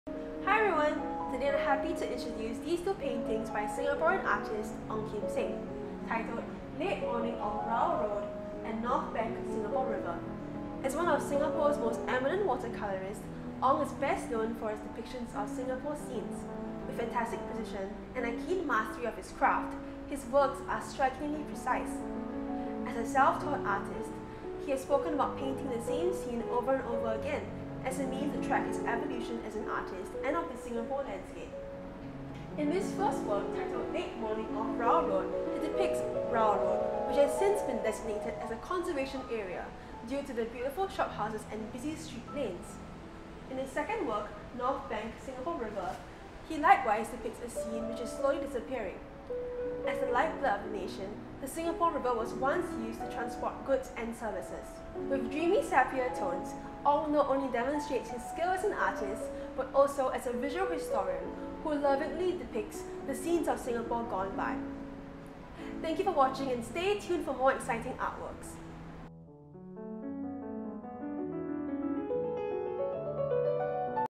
Art Introduction - 'Late Morning (Off Rowell Road)' and 'North Bank Singapore River' by Ong Kim Seng
We are happy to introduce these beautiful landscapes by Singaporean water colourist, Ong Kim Seng!